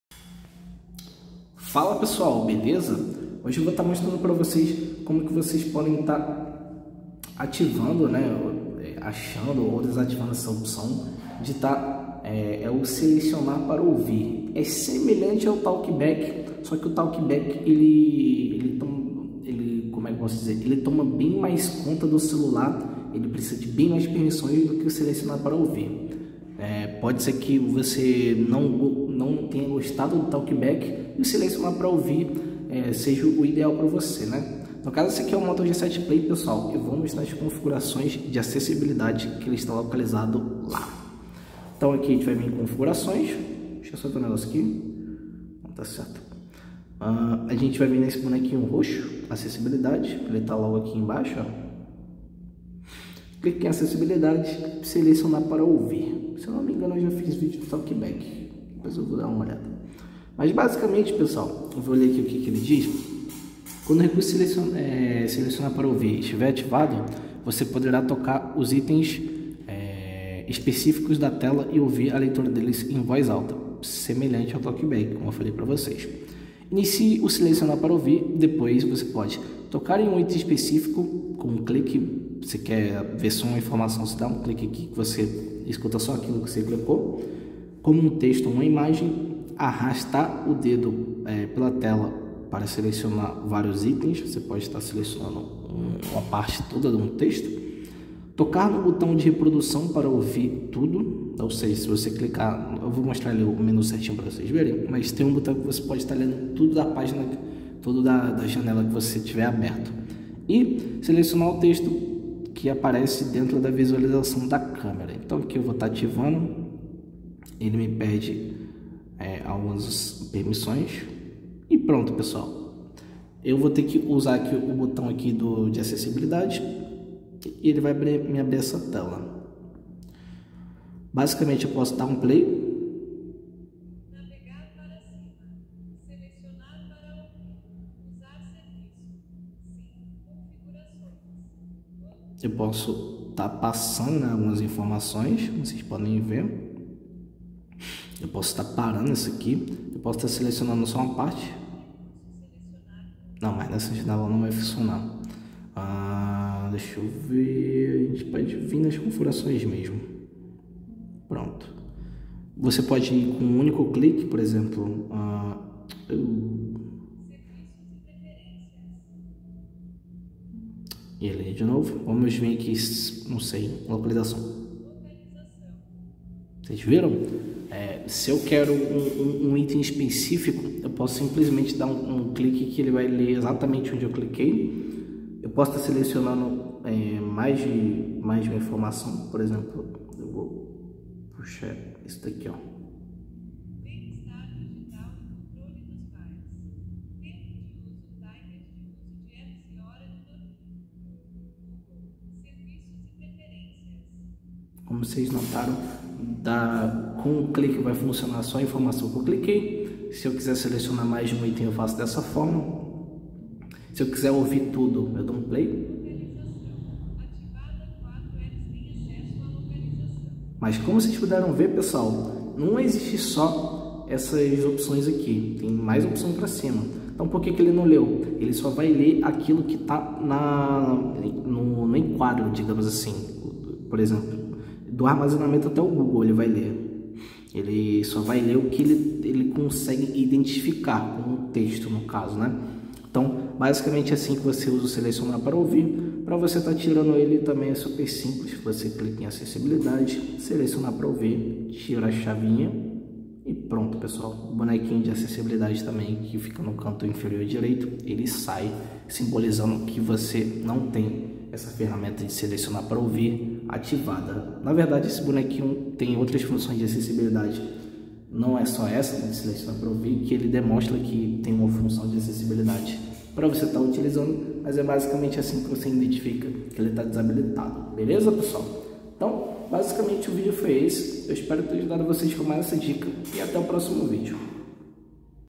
Moto G7 Play - Como ativar ou desativar o " Selecionar para ouvir "
No vídeo de hoje estarei mostrando como utilizar a função " Selecionar para ouvir " do menu de acessibilidade

✩ Redmi Note 9 64G / 128GB
✩ Redmi Note 9S 64G / 128GB
✩ Poco X3